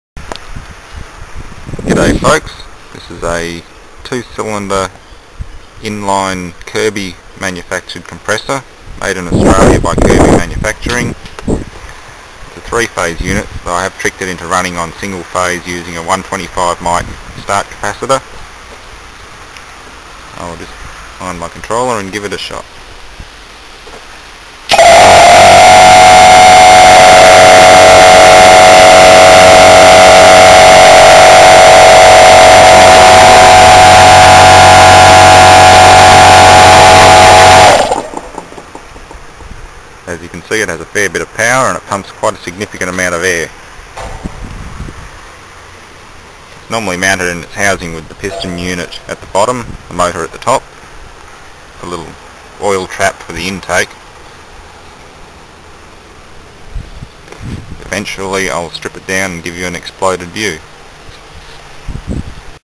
Kirby Reciprocating Commercial Fridge Compressor
This is a 2 cylinder Australian made refrigeration compressor, made by Kirby Manufacturing - Australia.

I removed it from a scrap condensing unit used on an IGA supermarket meatcase.

This one is similar to a 3 cylinder Kirby compressor that I scrapped 12 months ago, after it ran dry of oil (very little remained in crank case, mixed with melted plastic from the polypropylene splash shield) and subsequently overheated to the point of throwing the no2 con rod, jamming up and burning the motor windings out (no motor protector was fitted)